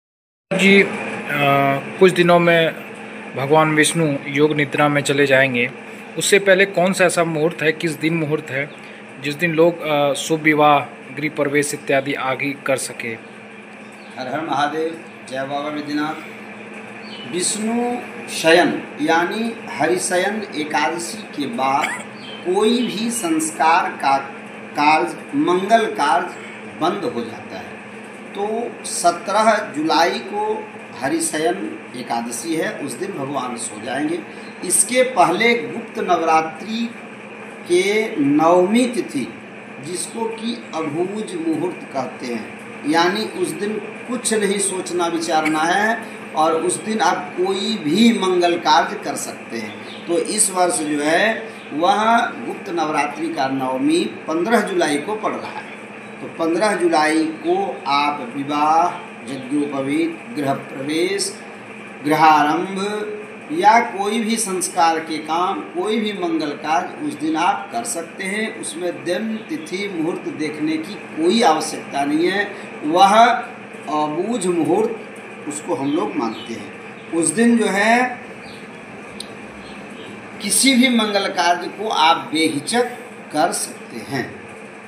Bhadli Navami 2024: कब और क्यों मनाई जाती है भड़ली नवमी? जानें मुहूर्त एवं योग
Giving information, astrologer of Deoghar, Pandit Nandkishore Mudgal said that the ninth date of Gupt Navratri, Abujh Muhurat, is considered similar to Akshaya Tritiya. On this day, all types of Mangalikkar can be done without looking at Nakshatra Tithi. Then after this you may have to wait for 4 months.

देवघर के ज्योतिषाचार्य पंडित नंदकिशोर मुद्गल ने जानकारी देते हुए कहा कि गुप्त नवरात्रि के नवमी तिथि अबूझ मुहूर्त अक्षय तृतीया के समान माना जाता है. इस दिन सभी प्रकार के मांगलिक्कर बिना नक्षत्र तिथि देखें कर सकते हैं.फिर इसके बाद 4 महीने आपको इंतजार करना पड़ सकता है.